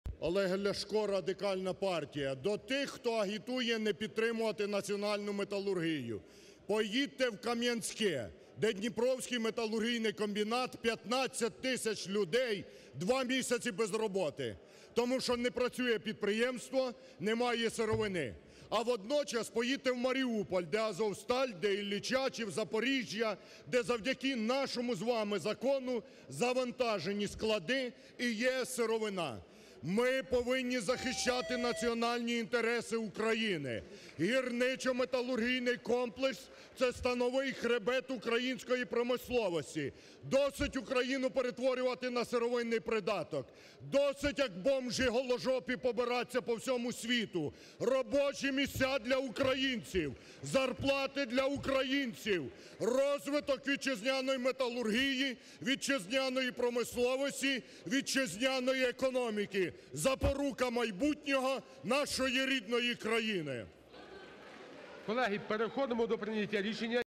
Ляшко: Досить побиратися по всьому світу, як бомжі голожопі!
Верховна Рада підтримала законопроект Радикальної партії про продовження на 2 роки дії підвищеного до 30 євро за тону мита на експорт металобрухту.
«Ті, хто агітував не підтримувати національну металургію, проїдьтеся  по українських підприємствах. Зокрема, на Дніпровському металургійному комбінаті 15 тисяч людей сиділи без роботи. Підприємство не працювало, бо не було сировини. Завдяки нашому закону про мито на металобрухт воно завантажено сировиною і працівники на таких заводах, як «Азовсталь», «Меткомбінат» та інші по всій країні, мають роботу і зарплату», - підкреслив лідер РПЛ Олег Ляшко.
Політик закликав колег-нардепів захистити вітчизняну промисловість.
«Гірничо-металургійний комплекс – це слоновий хребет економіки України. Потрібно підтримувати вітчизняного виробника. Досить країну перетворювати на сировинний придаток і побиратися по всьому світу, як бомжі голожопі!», - наголосив народний депутат.